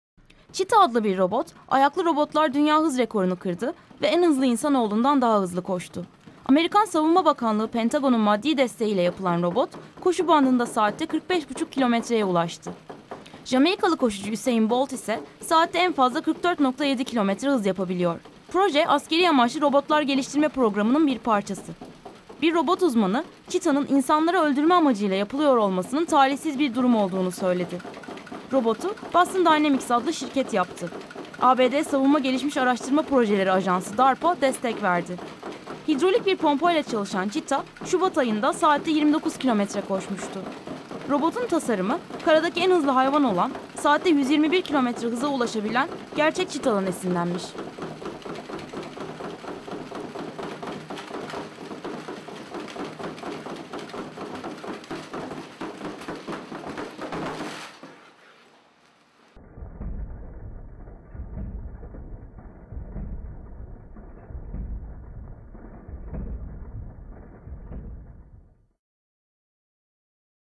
Usain Bolt'dan hızlı koşan robot yapıldı
'Çita' adlı ayaklı robot, en hızlı insanoğlunu geçerek dünya rekoru kırdı. Pentagon Çita'nın 'askerlere yardımcı olacağını' söylüyor.